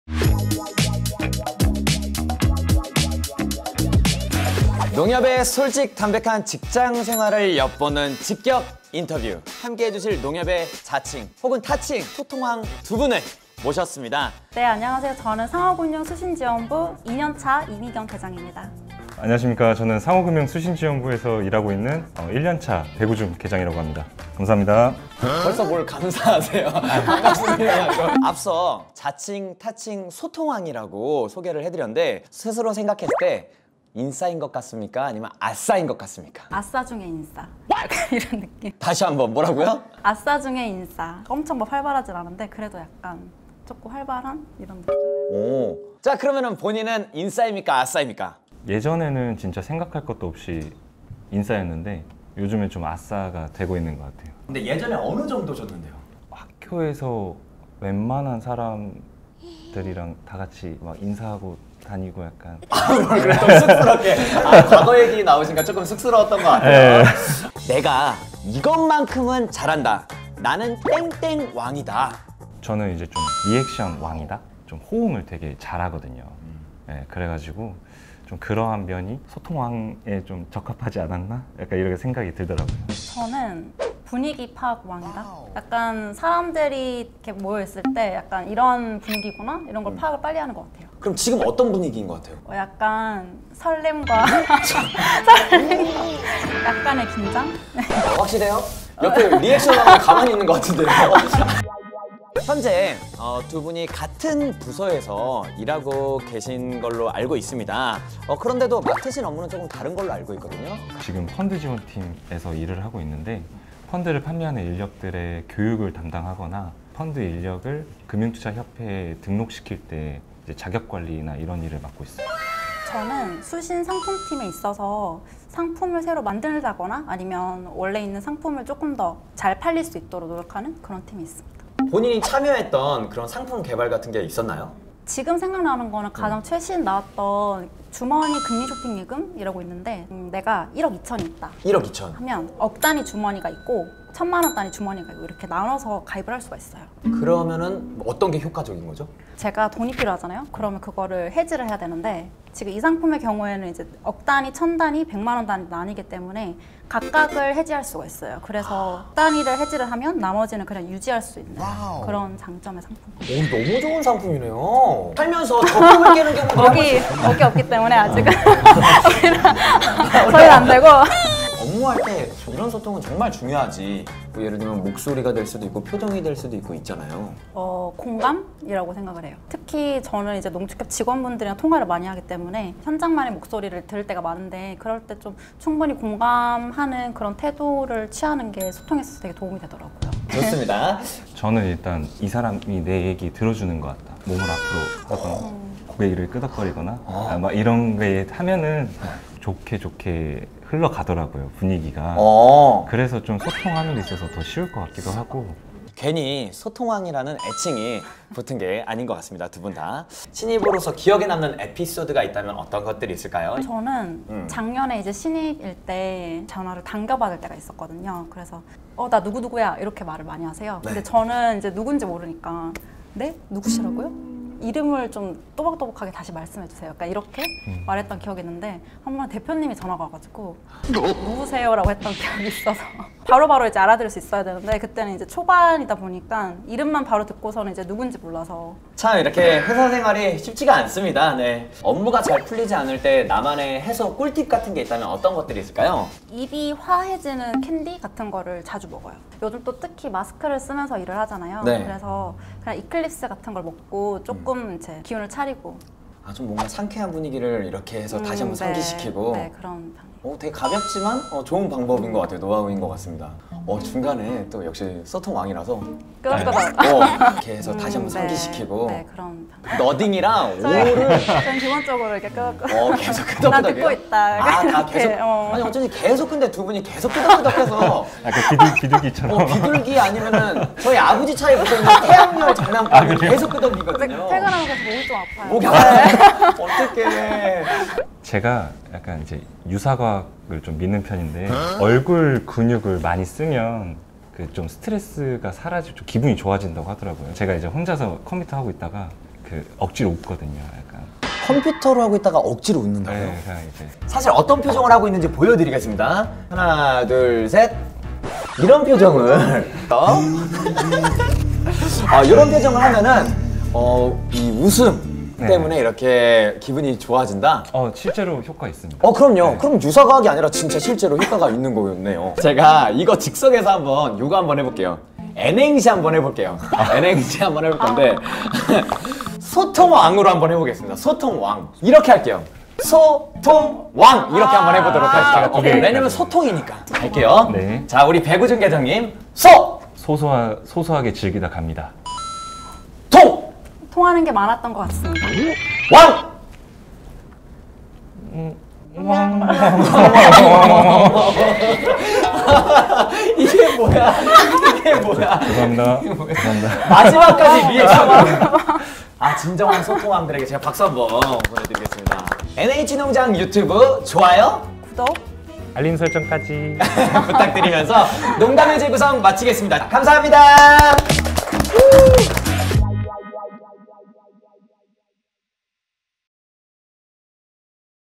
[농담의 재구성] 직장인의 디폴트 값은 ‘끄덕임’과 ‘Oh’다
오고가는 농담(弄談) 속 숨어있는 진담(眞談)💰
NH농협 직원들의 솔직 담백 인터뷰, 농담의 재구성!

직장인이라면
끄덕임과 '넵!’, ‘오(Oh)!’는 필수다 ❗❓

자칭 타칭 소통왕 두 사람의 신의 한수 소통 꿀팁🍯부터
신입시절 심장 떨리던 실수담까지
궁금하다면! 지금 만나보세요🤗!

00:10 인터뷰 시작🎬과 함께하는 자기소개
00:36 인싸인듯 아싸인듯 인싸같은 너~~~
02:18 적금 깰 일이 있다? 동작그만🖐️!
03:11 (주목👀)소통과 리액션의 기본값 푼다!
03:32 싸늘하다. 🌬️ ‘누구세요’가 이렇게 떨리는 말이었던가...
04:24 퇴근할 때 목이 뻐근하다? 그렇다면 당신은 소통왕왕왕왕왕
05:30 디폴트 값에 없던 삼행시